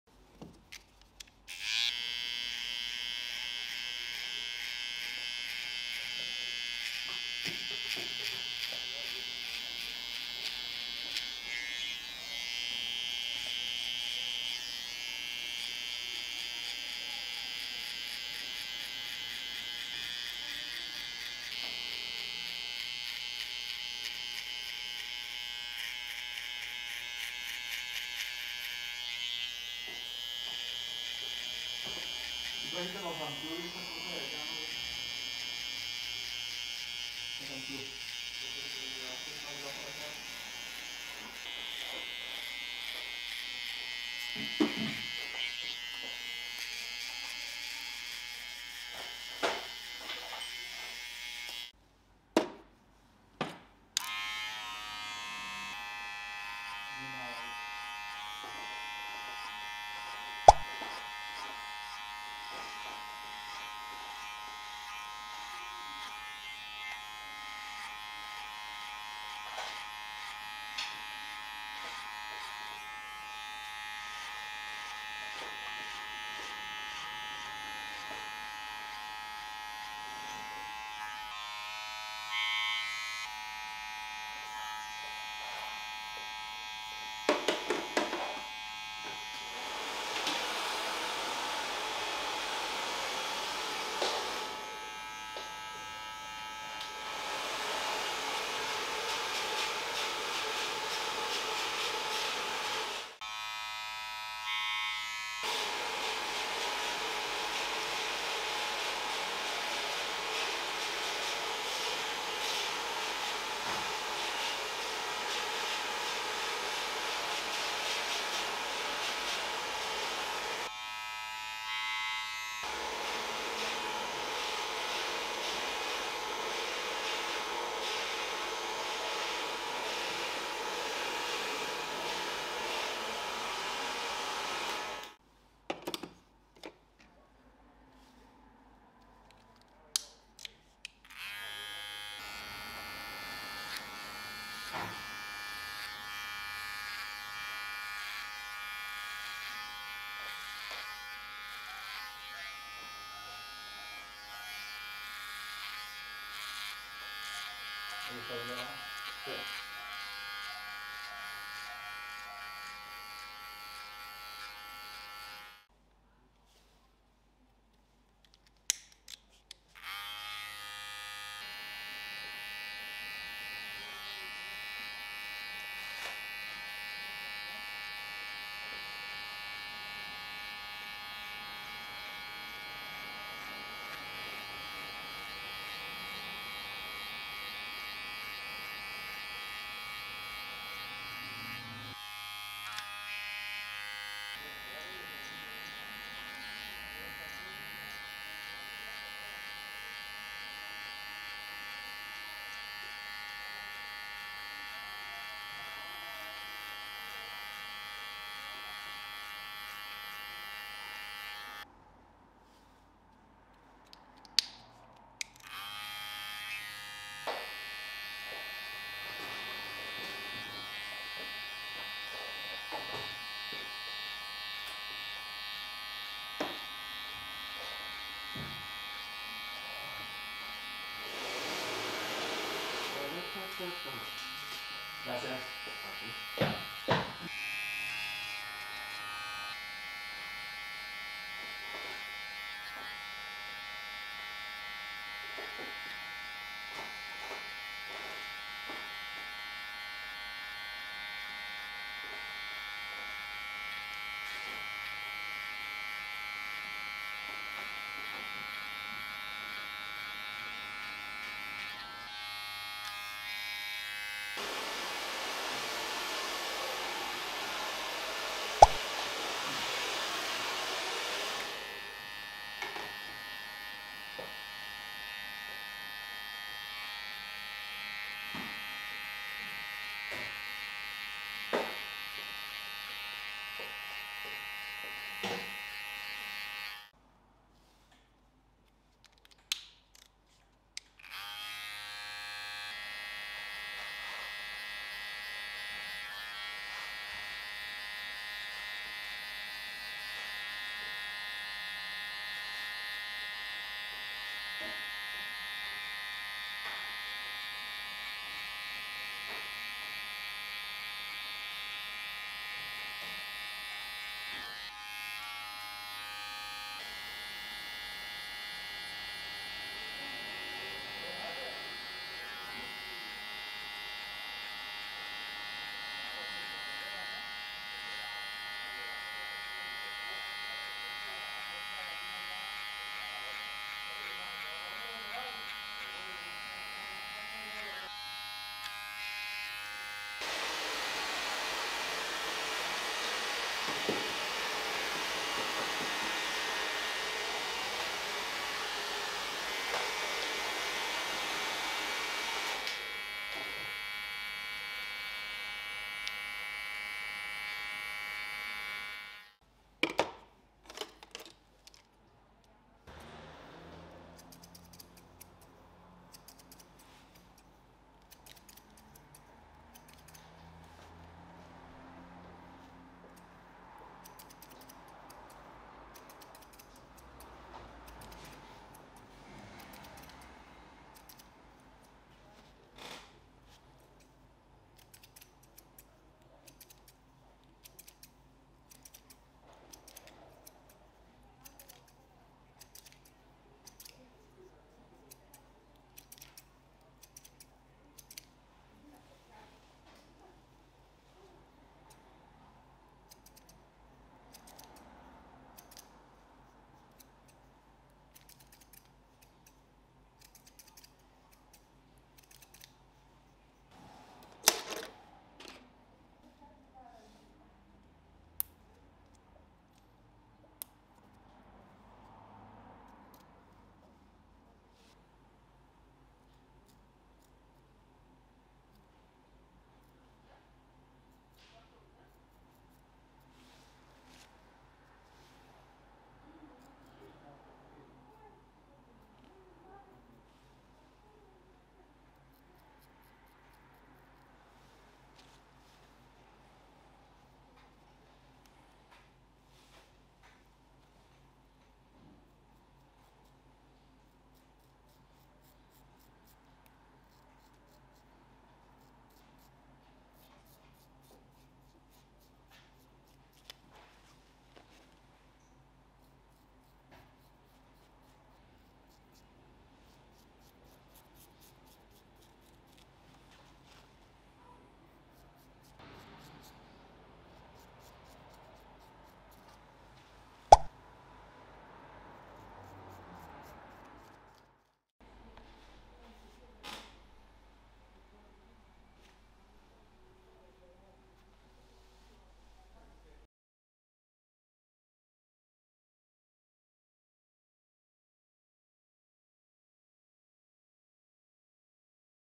ASMR HAIRCUT - how to make the perfect SKIN FADE step by step - Barber Tutorial 🟡
Asmr haircut Barber shop tutorial how to do a fade step by step